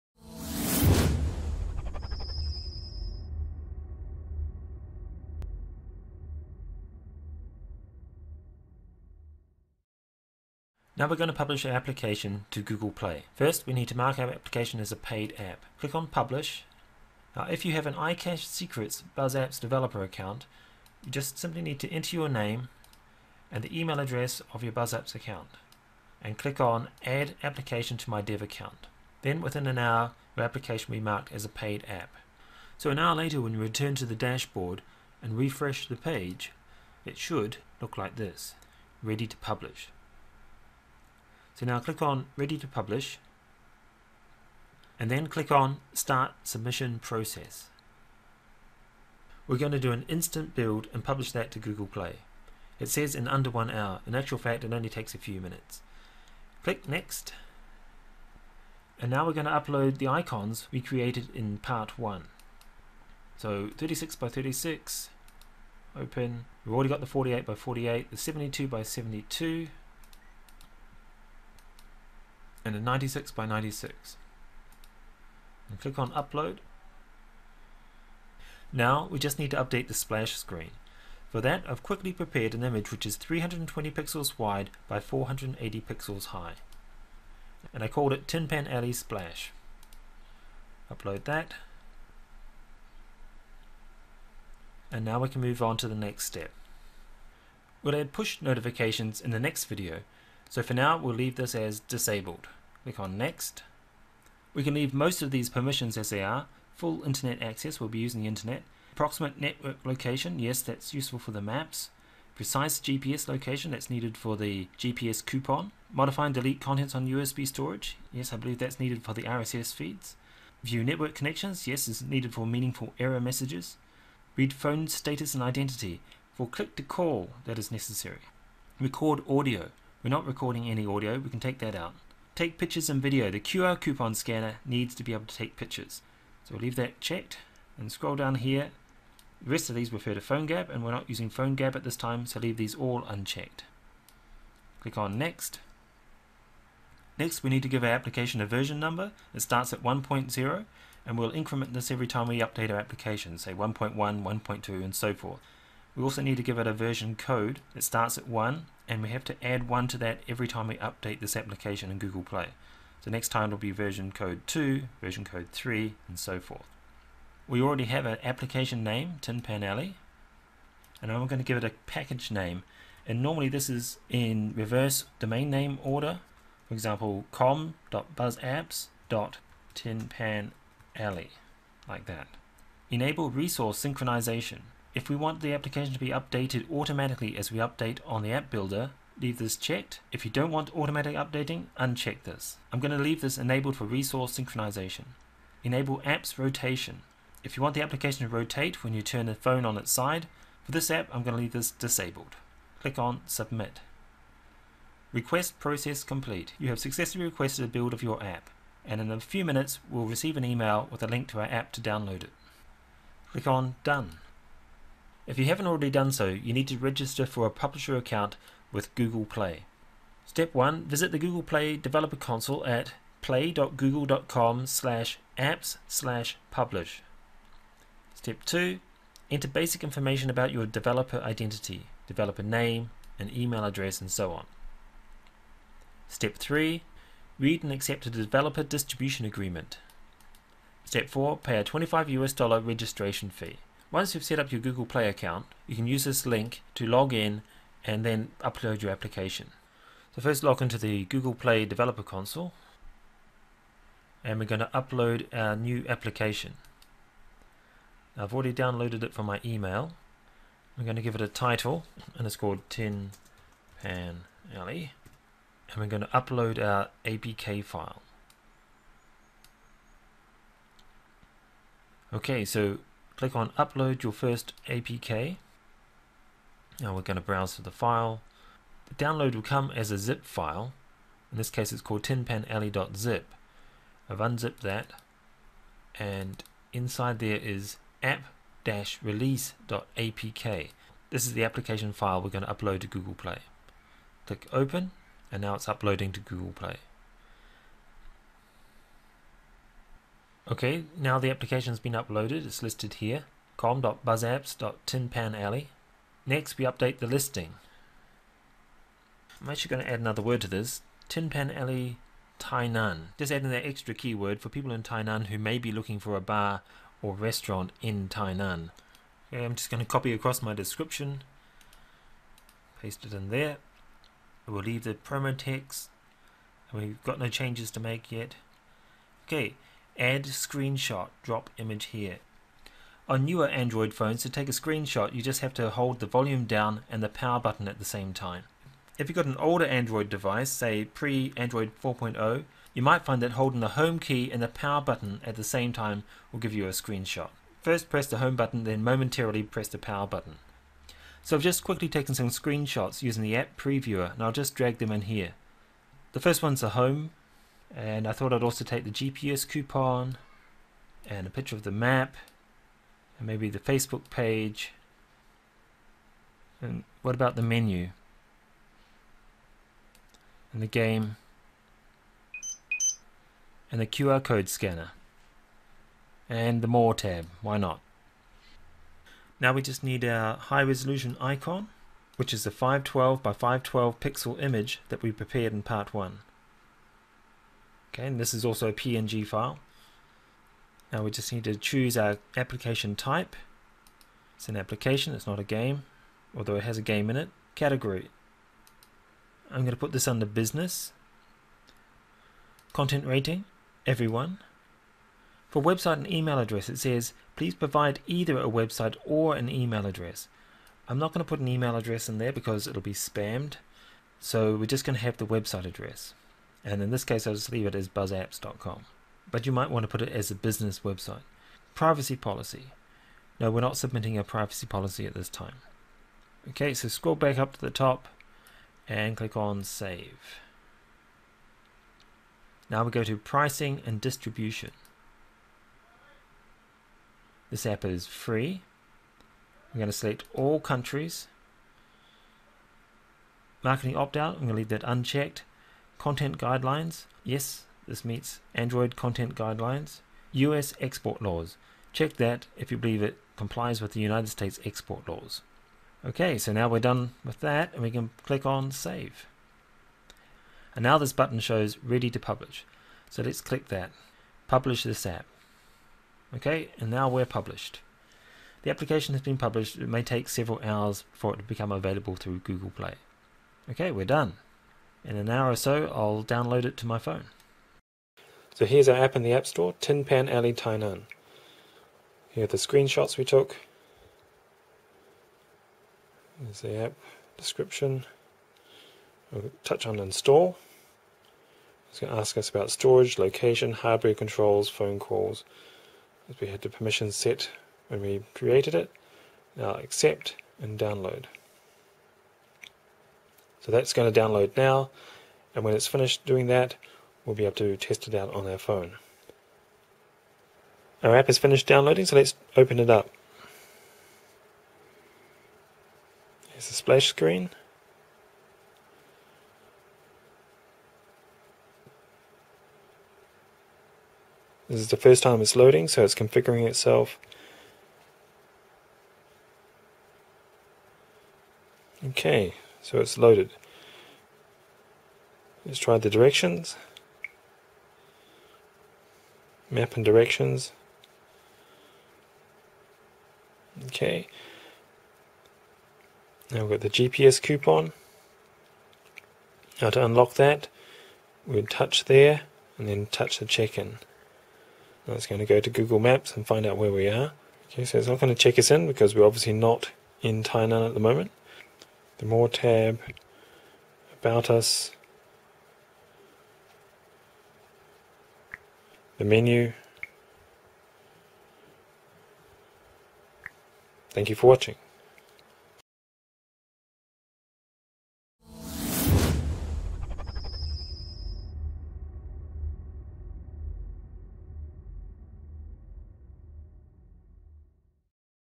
Building A Restaurant Bar App - Part 3 - Publishing Your App to Google Play
Mobile App Builder for iOS, Android and Webapps.
Discover how easy it is to get started as a pro app developer

In this video we are going to publish our Restaurant Bar application to Google Play.